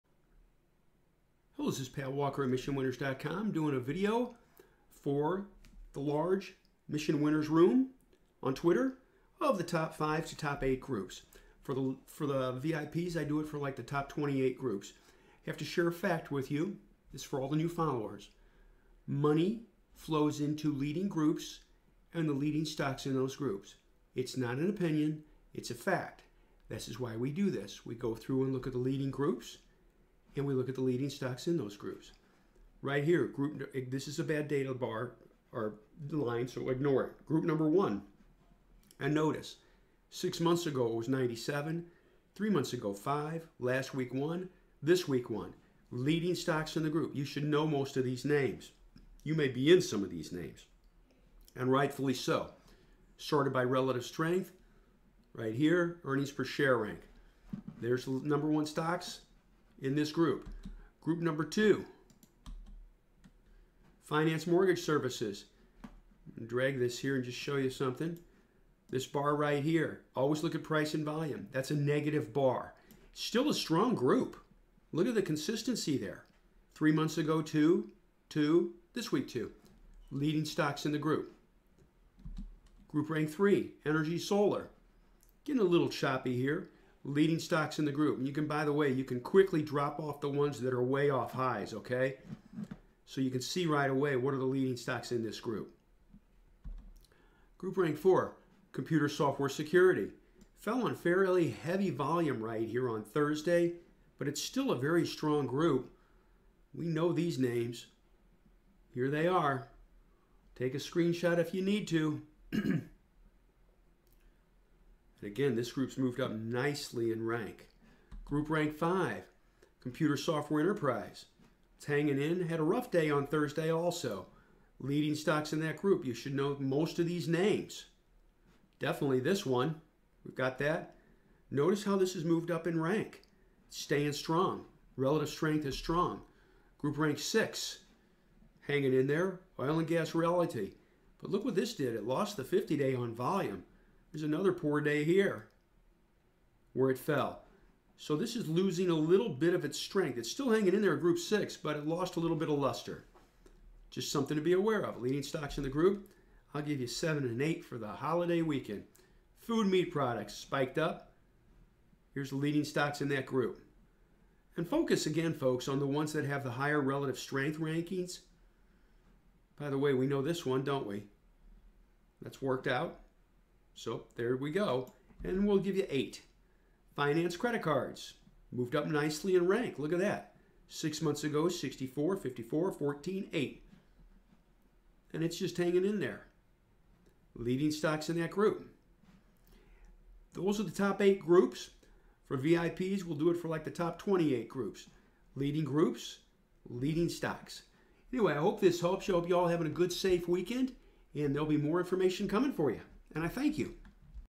2019 05 26 Top Stocks and the Top 5 Groups
Top 5 Leading Groups:

You and I can't move a stock's price. We rely on knowing into which stocks the large institutions are investing money. That is where determining Leading Industry Groups comes into play.

It's important to stay abreast of which industry groups are currently in the top groups as institutional money will typically flow into those top industry groups.

More info at MissionWinners.com